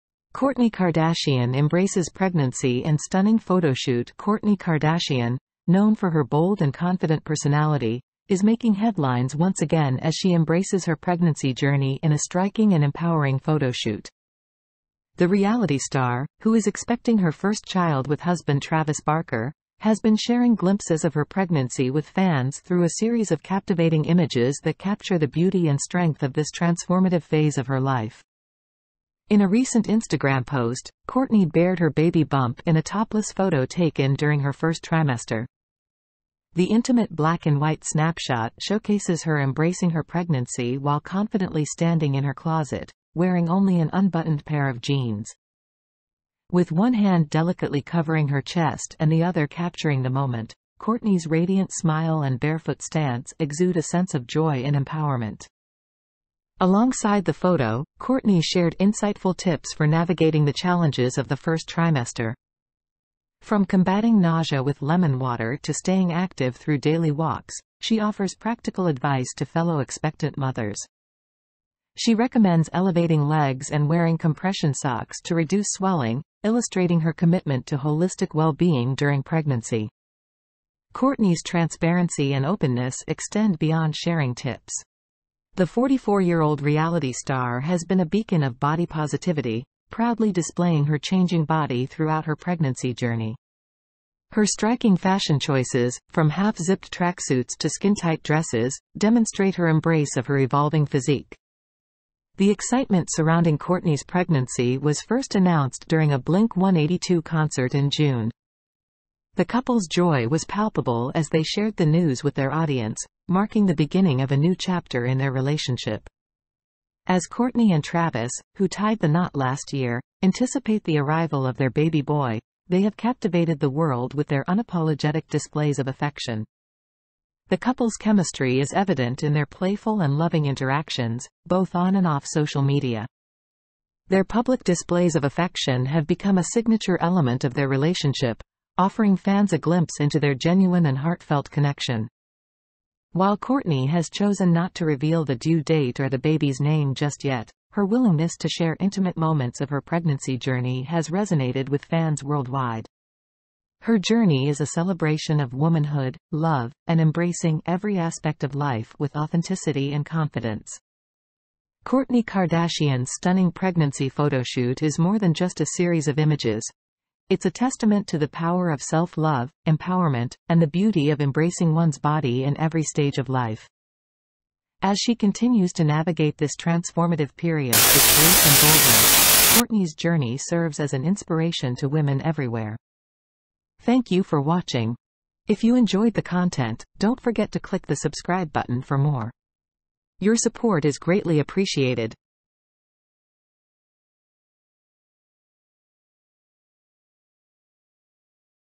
The Power of Self-Love: Kourtney Kardashian's Captivating Pregnancy Shoot
Kourtney Kardashian's Pregnancy Photoshoot Is Absolutely Stunning
Kourtney Kardashian, known for her outgoing and self-assured nature, is making headlines once again for a striking and inspiring picture in which she celebrates her pregnancy. The reality actress has been giving fans a sneak peek at her pregnancy through a series of stunning photographs that highlight the beauty and power of this remarkable time in her life.
Kourtney recently posted a topless selfie of herself pregnant on Instagram. The shot was likely taken somewhere in the first trimester. The private black-and-white photo shows her proudly accepting her pregnancy while standing in front of her closet door with her jeans unzipped. Kourtney's dazzling smile and barefoot stance emanate a sense of excitement and empowerment as she stands with one hand carefully covering her chest and the other filming the moment.
Kourtney also included some helpful advice for dealing with the difficulties of the first trimester of pregnancy. She gives helpful suggestions to other new women, such as drinking lemon water to prevent sickness and going for daily walks to stay active. As an example of her holistic approach to health throughout pregnancy, she suggests elevating the legs and using compression stockings to minimize edema.
Kourtney's candor and openness aren't limited to the realm of offering advice. The 44-year-old reality actress has been an example of body positivity by sharing photos of her growing baby bump on social media. Her daring outfit choices, which range from skintight skirts to half-zipped tracksuits, show that she is confident with her changing body.
Kourtney's pregnancy was announced to much fanfare at a June Blink-182 show. The audience could feel the couple's excitement as they announced the beginning of a new phase in their relationship.
Kourtney and Travis, who married last year and are expecting a son, have been captivating the public with their unabashed displays of passion in recent months. The couple's chemistry shines through in their affectionate and playful exchanges, both online and off. Their public displays of affection have become a hallmark of their relationship, giving followers a look into the real and affectionate bond they share.
Fans all around the world have been touched by Kourtney's decision to share private moments from her pregnancy journey, even though she has yet to announce the due date or the baby's name. Her adventure is a triumphant ode to womanhood, love, and total self-possession in the face of any challenge.
Kourtney Kardashian's lovely pregnancy photoshoot is more than simply a collection of pictures; it's a celebration of the importance of loving and accepting one's body at every age and stage. Kourtney is a role model for women everywhere as she continues to show courage and poise as she faces this pivotal time in her life.